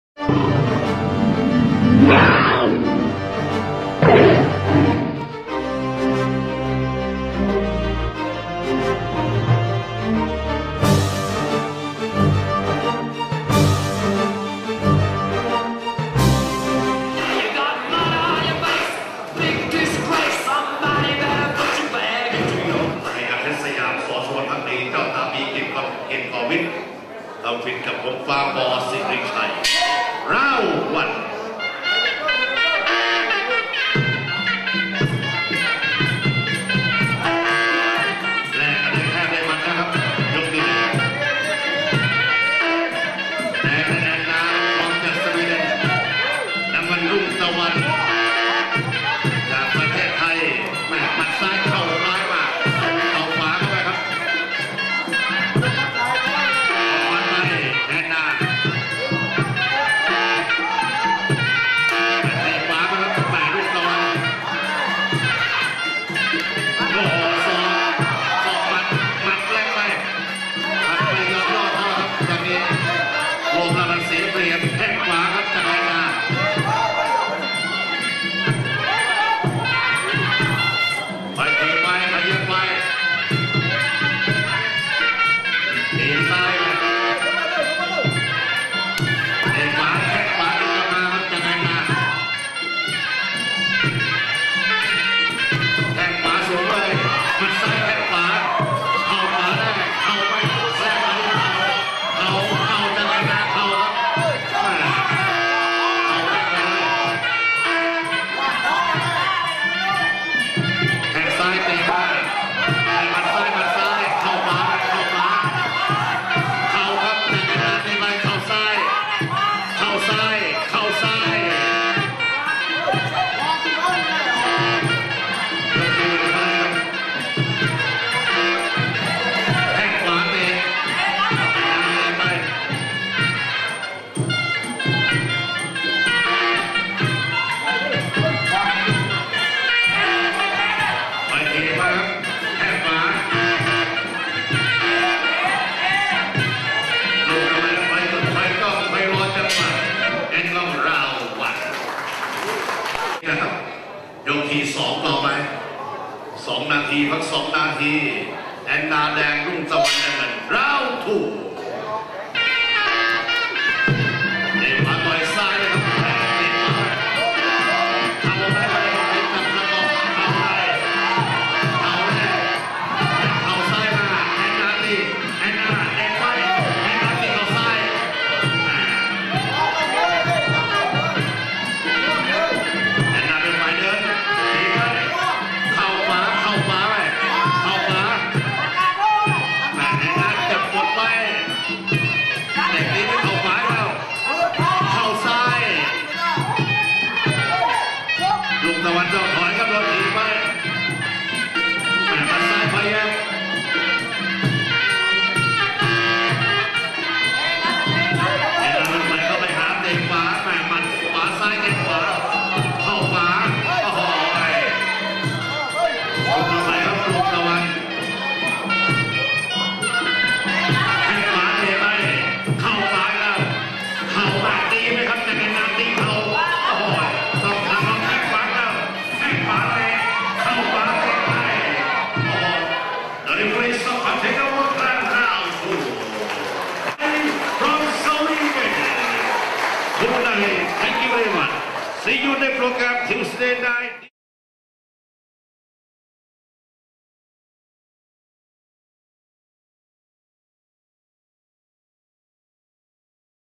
Anna (Tiger Muay Thai) scores a 2nd round KO at Chaweng boxing Stadium Koh Samui
Anna fighting out of Tiger Muay Thai & MMA training camp, Phuket, Thailand, defeats Roog Tawan (Thailand) at Chaweng Stadium, Koh Samui on November 27, 2009.